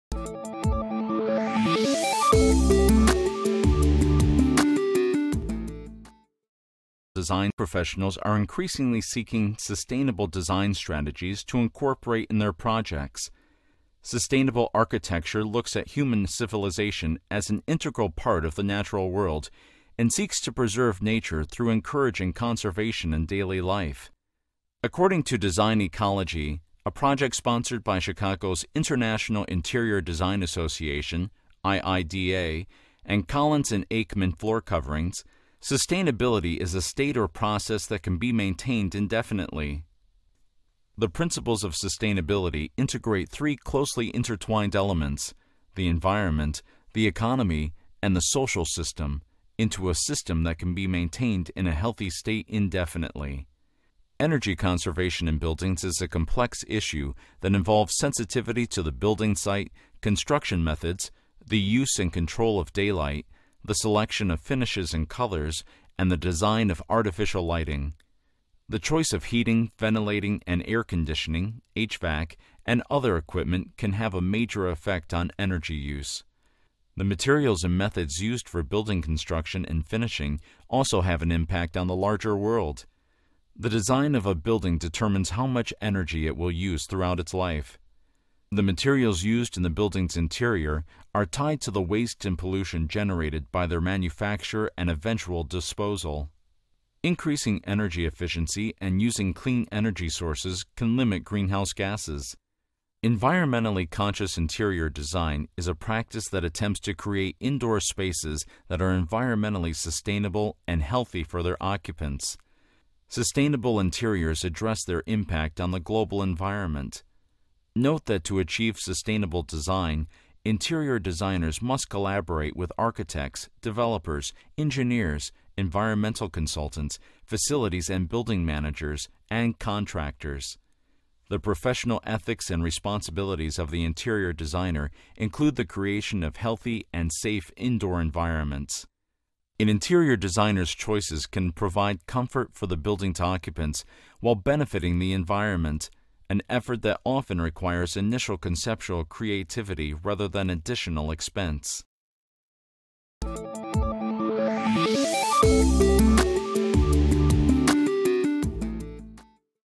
Sustainable Design Strategies
Design professionals are increasingly seeking sustainable design strategies to incorporate in their projects.

This RedVector course, Building Systems for Designers: The Building and Its Environment examines the connection between a building's interior & exterior environment and it's sustainable design strategies in addition to building codes and regulations.